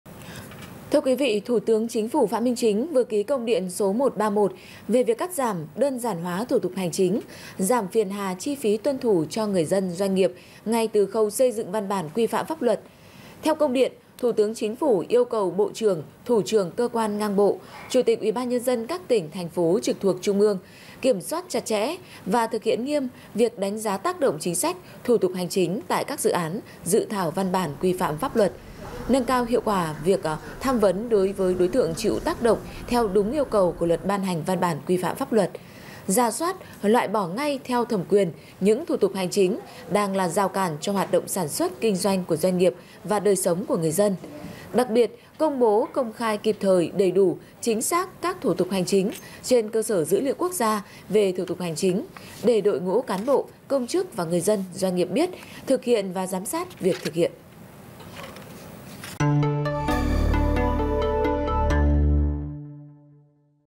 Thủ tướng Phạm Minh Chính vừa ký Công điện yêu cầu cắt giảm, đơn giản hóa thủ tục hành chính - VNews
VNews -  Thủ tướng Chính phủ Phạm Minh Chính vừa ký Công điện số 131/CĐ-TTg ngày 11/12/2024 về việc cắt giảm, đơn giản hóa thủ tục hành chính, giảm phiền hà, chi phí tuân thủ cho người dân, doanh nghiệp ngay từ khâu xây dựng văn bản quy phạm pháp luật.